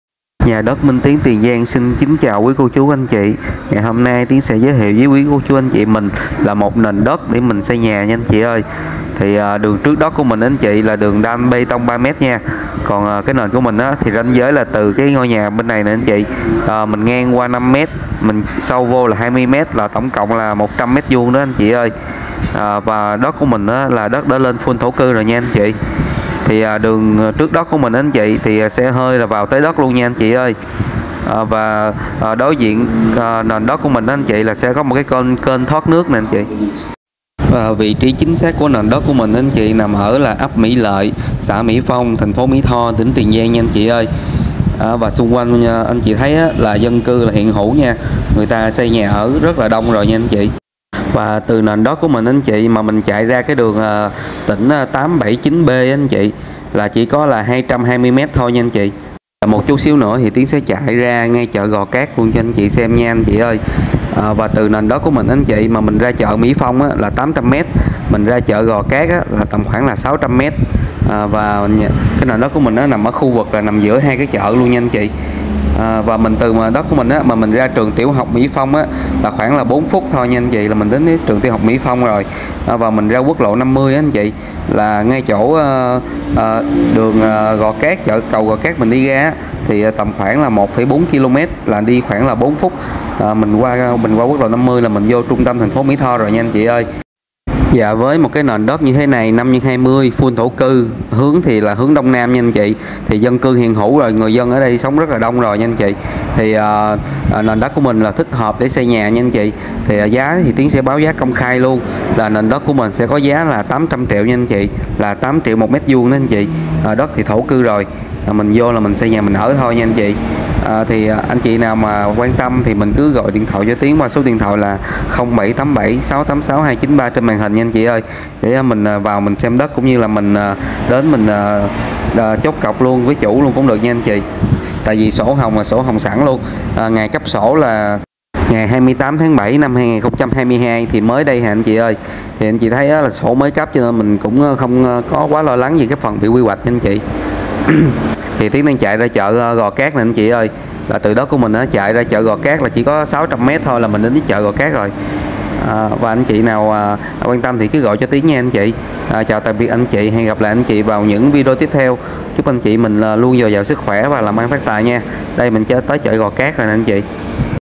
TẬP 94:BÁN ĐẤT XÂY NHÀ HOẶC MUA ĐỂ DÀNH. 5X20 FULL THỔ CƯ. MỸ LỢI, MỸ PHONG, MỸ THO, TIỀN GIANG. 800
🔥🔥 Mã số 94
🌈  BÁN ĐẤT ĐƯỜNG XE HƠI MỸ THO.
VỊ TRÍ: Ấp Mỹ Lợi, Xã Mỹ Phong , Mỹ Tho  ,Tỉnh Tiền Giang.
📐 Diện Tích :5x20 100m2  FULL THỔ
⚖ Pháp Lý : Sổ Hồng Riêng. Cấp ngày 28/07/2022
⛽🏥🏦🏨 Tiện ích: ĐƯỜNG BÊ TÔNG XE HƠI TỚI ĐẤT. CÁCH TL879B Chỉ 220M. Nằm giữa 2 khu chợ là CHỢ MỸ PHONG 800M VÀ CHỢ GÒ CÁT 600M. GIAO THÔNG THUẬN LỢI RA QUỐC LỘ 50 VÀO TRUNG TÂM MỸ THO CHỈ 4 PHÚT.
NHẬN KÝ GỬI MUA BÁN NHÀ ĐẤT TẠI TIỀN GIANG.